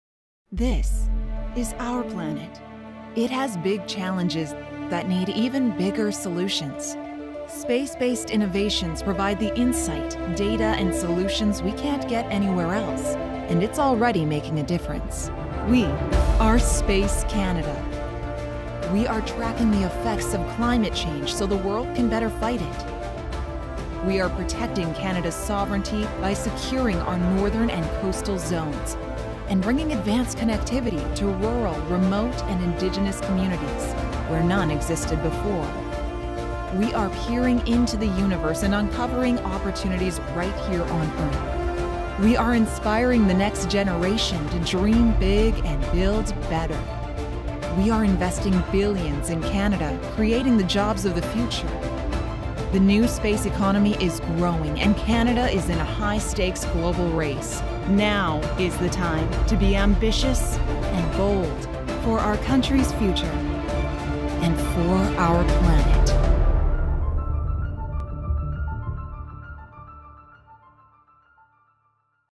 Space Canada
Canada's Space Innovators | Les innovateurs de l’espace du Canada

Canadian space innovation is solving today's biggest challenges on Earth and in space. And in order to compete in the global space race, we are investing, scaling and creating the jobs of the future. The time is now to invest in space, Canada.